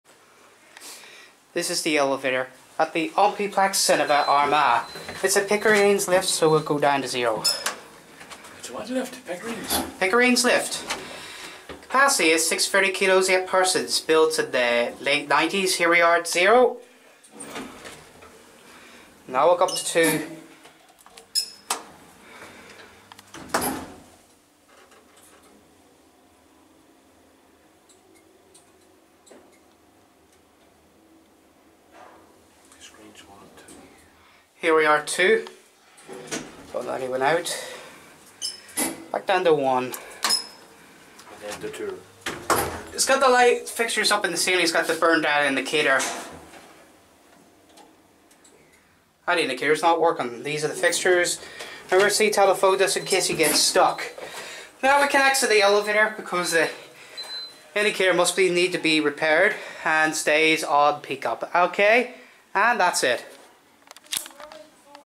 Pickerings Elevator At The Omniplex Armagh
pickerings elevator at the omniplex 3 Floors (0, 1, 2,)